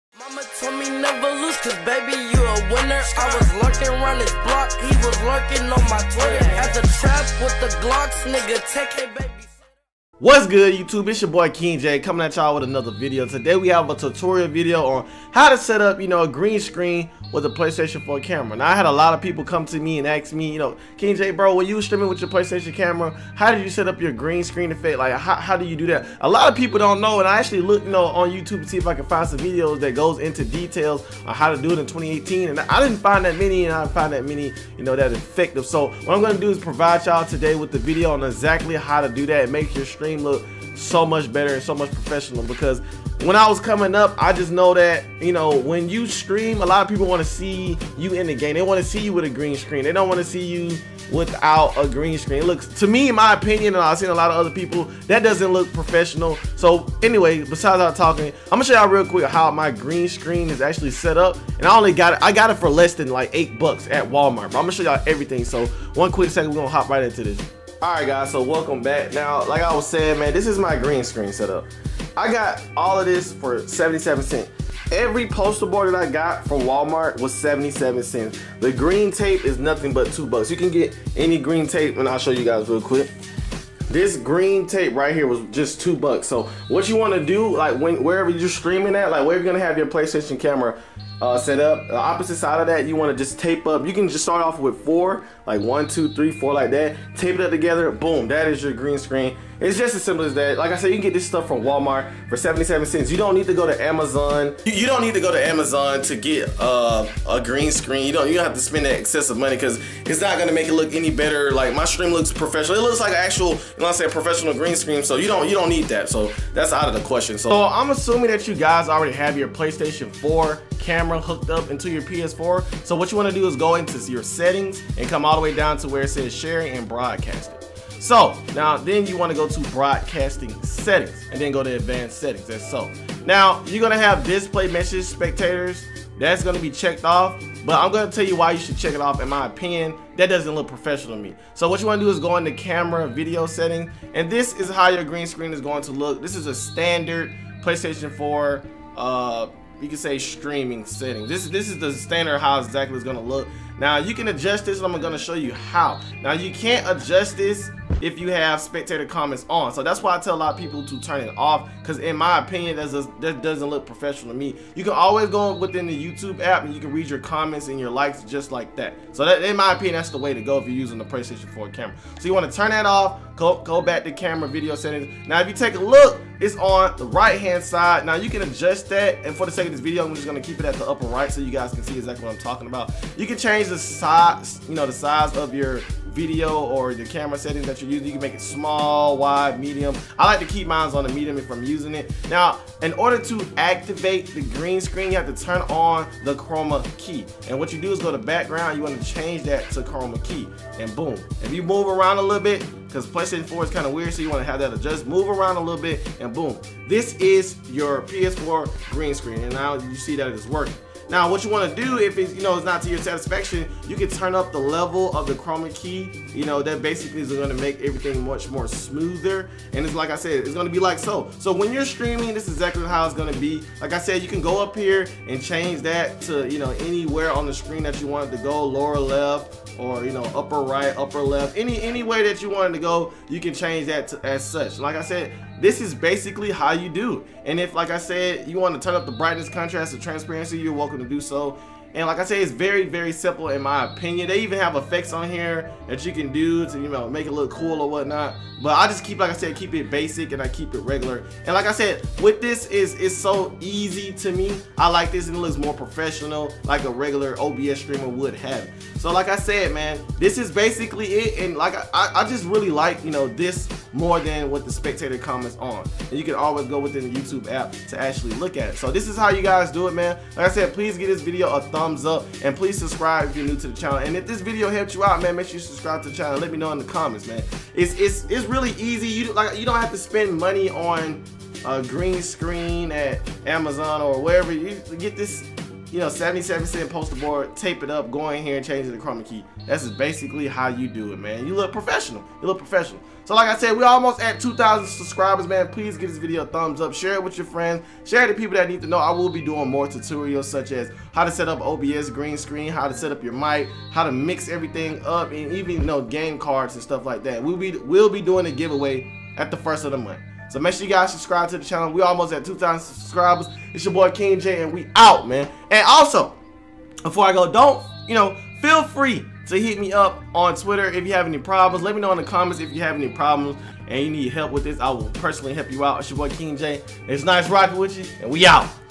HOW TO SETUP GREEN SCREEN WITH PS4 CAMERA(2020)/PS4 STREAMING TIPS!
How to setup green screen with ps4 camera 2020 and also ps4 camera streaming tips! In this tutorial i show ps4 users how to set up the chroma key green screen and set it up for live streaming. This tutorial will make your ps4 streams look like obs green screen settings! It is very easy and up to date . Tip! With these settings your ps4 live stream will look like prettyboyfredo and cashnasty live stream!

PSN -- AS1AS2